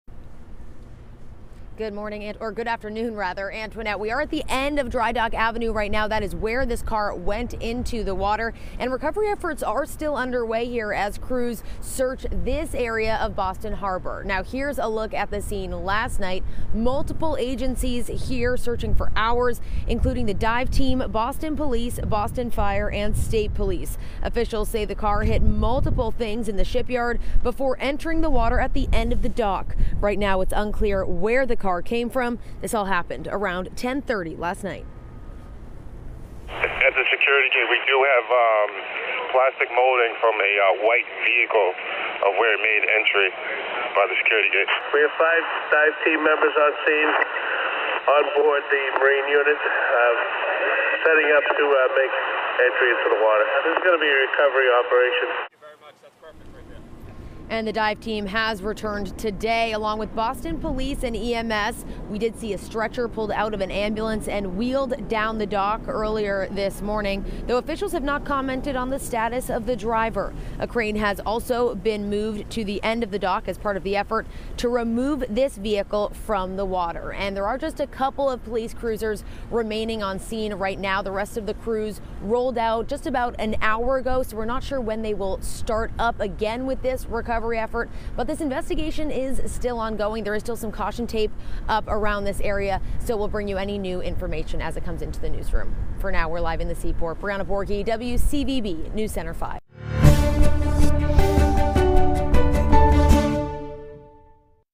Vehicle crashes into Boston Harbor
Officials are working to remove a vehicle that crashed into Boston Harbor.